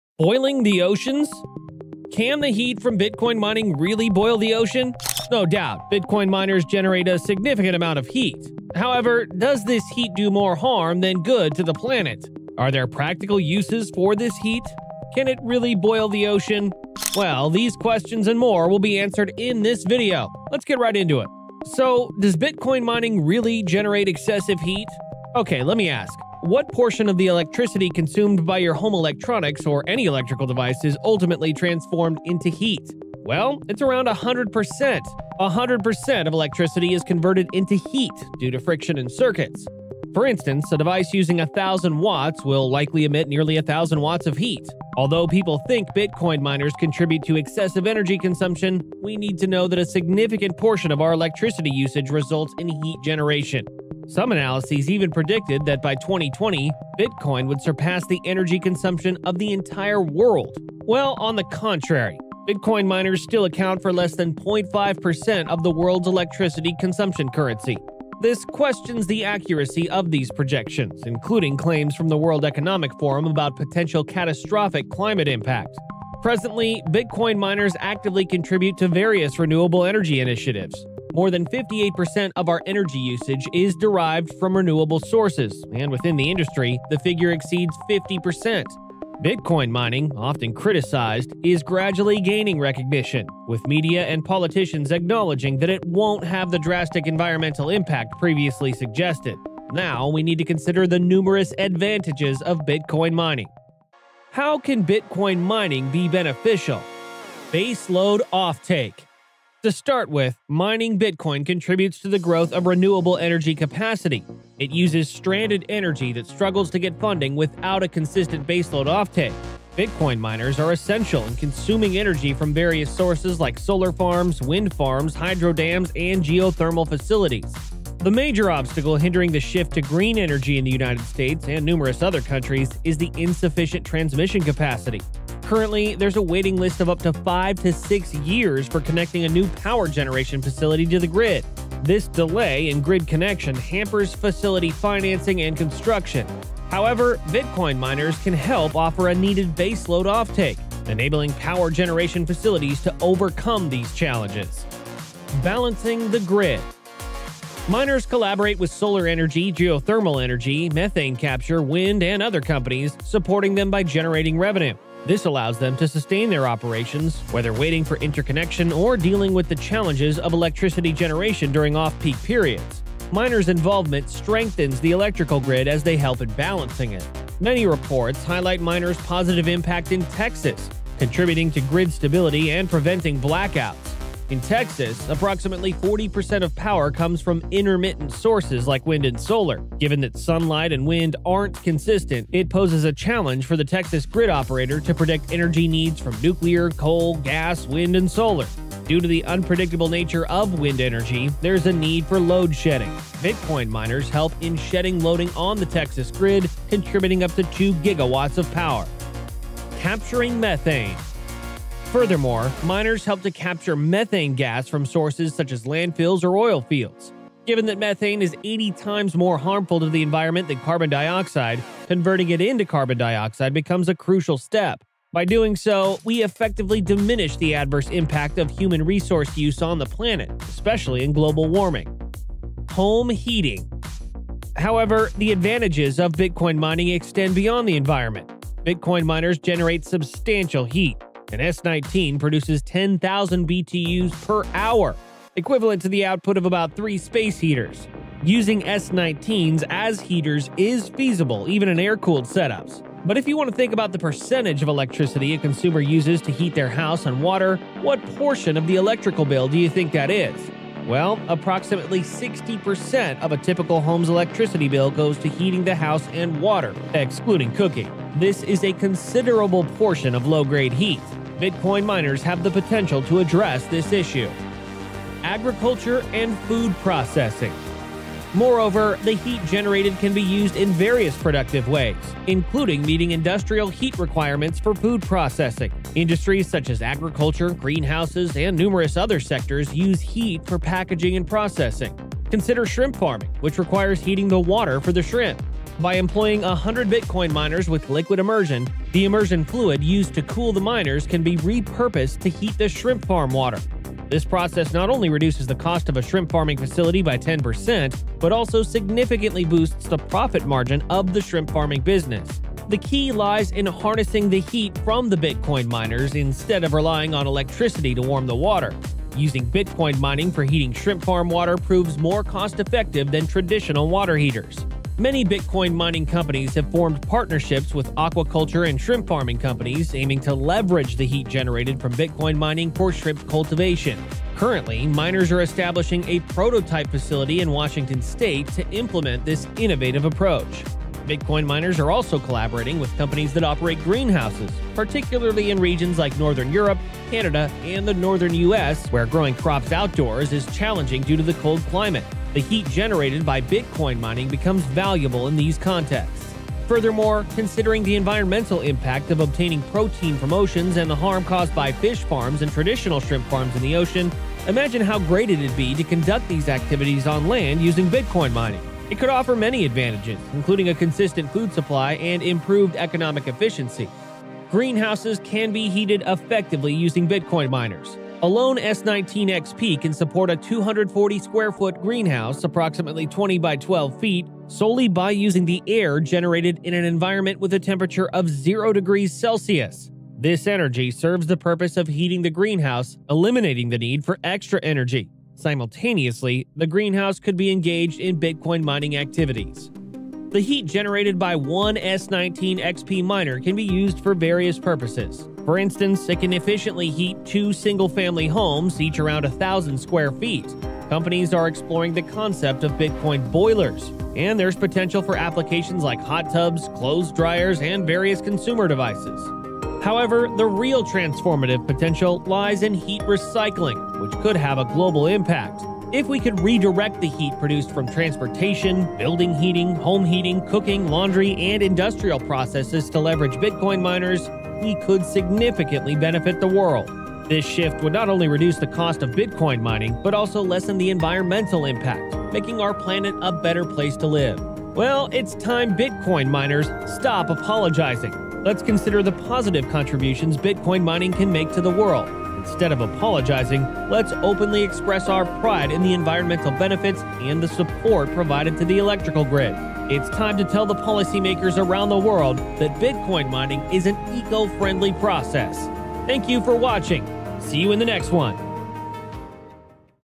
The Shocking Truth: Can We Really Boil the Oceans?
Can the heat from Bitcoin mining really boil the ocean?  No doubt, Bitcoin miners generate a significant amount of heat. However, does this heat do more harm than good to the planet? Are there practical uses for this heat? Can it really boil the ocean? Well, these questions and more will be answered in this video. Let's get right into it!